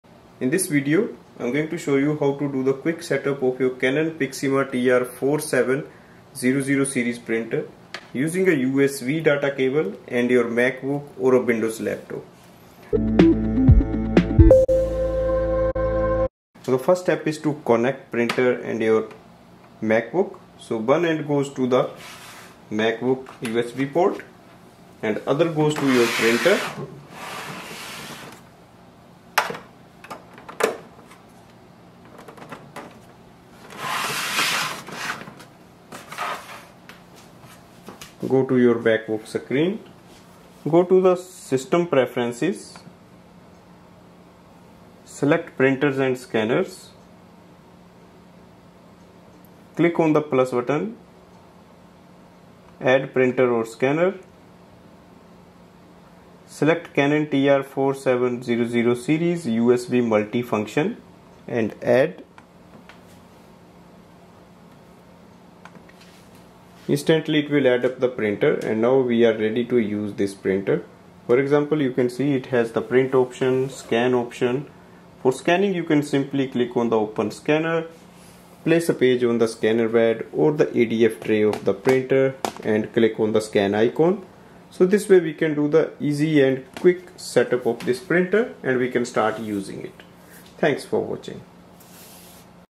Canon Pixma TR4700, 4720, 4722, 4723, 4750i, 4751i Quick USB Setup | Fast Setup, Printing & Scanning
This video reviews how to do the quick USB Setup with the Canon Pixma TR4720, TR4722, TR4723, TR4750i and TR4751i All-in-one inkjet printer for fast printing and scanning.